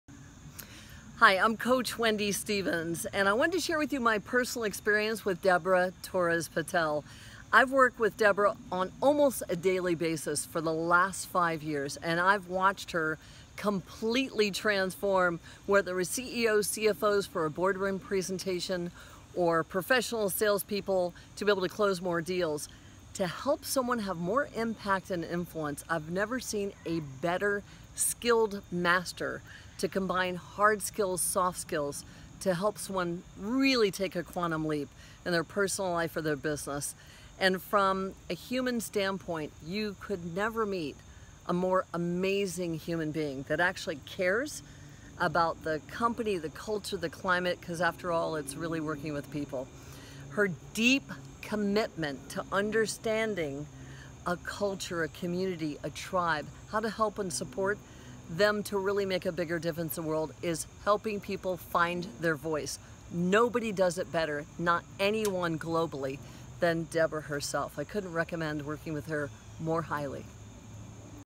Coach Wendy Stevens Endorses Deborah Torres Patel
Deborah Torres Patel, International Public Speaking, Voice & Presentation Skills Expert is Endorsed by Guerilla Marketing Expert Wendy Stevens